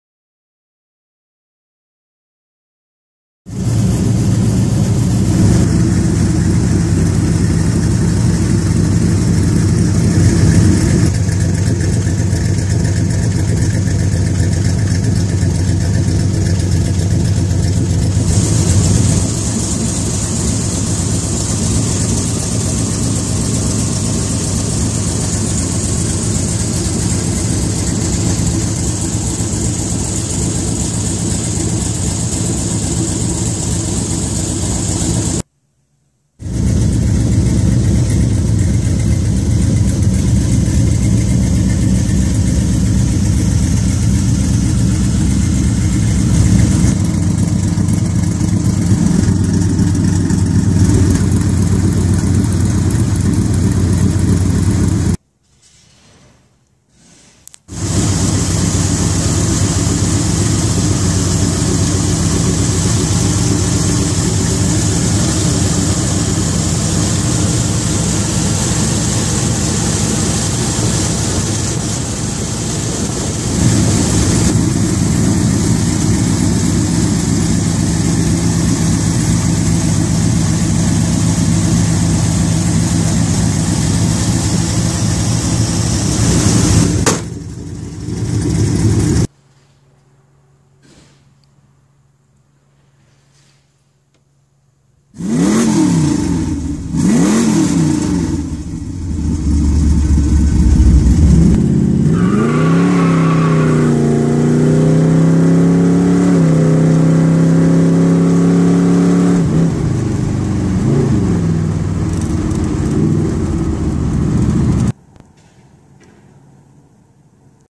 Full Race Autocross C3 Corvette - "Chiquita"
My 1977 Corvette set up for Autocross. 100% Forged, 100% Roller, 500HP 500ft Lbs Torque, Edelbrock, Dart Heads, Eagle Crank, Howard's Cams, Hurst Shifter, B&M Racing T4 Trans, Hughes Stall Converter, Quickfuel Carb.  Lowered 2"s, Bilstein shocks, Carbon Fiber rear spring, all Gas, Spark and Air... 22 Wins and 0 Losses on the street ... so far... and it is not set up for drag racing!  Mostly original paint she is not a Survivor, she is the Victor!